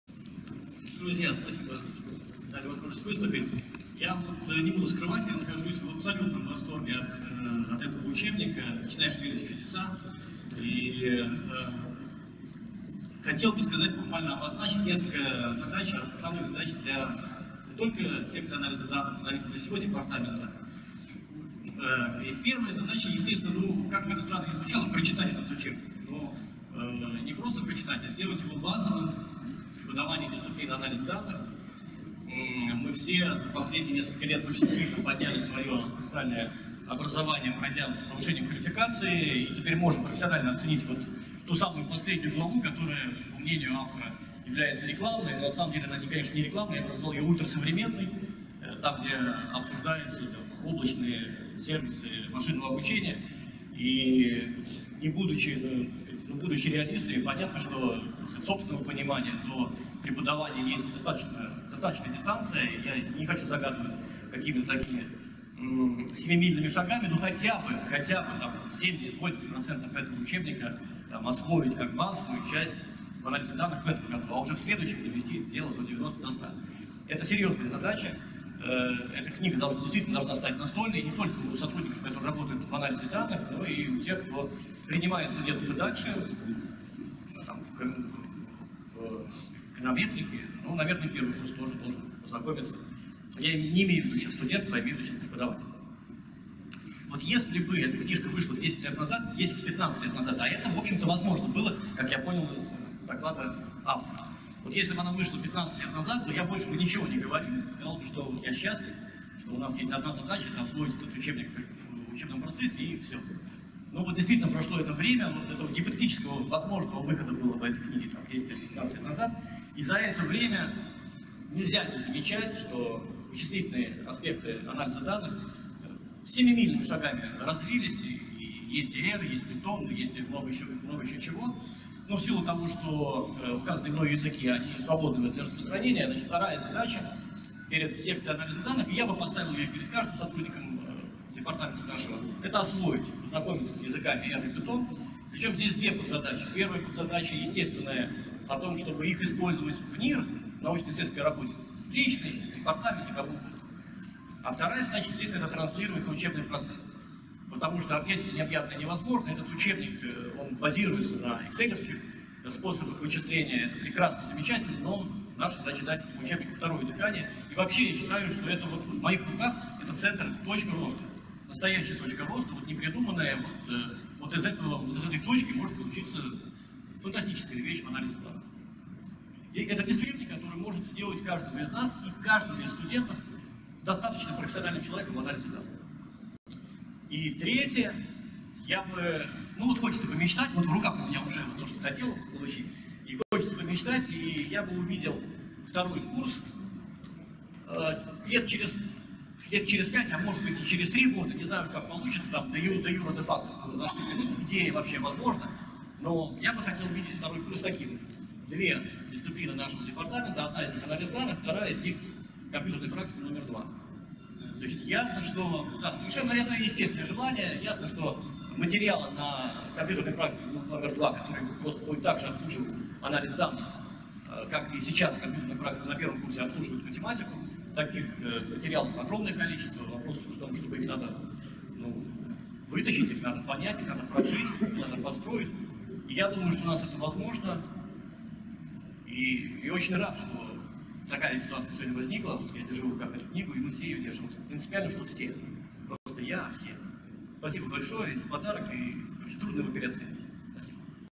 Презентация учебника «Анализ данных в экономике» - Выступление С.А. Зададаева
Презентация учебника «Анализ данных в экономике» - Выступление Сергея Зададаева.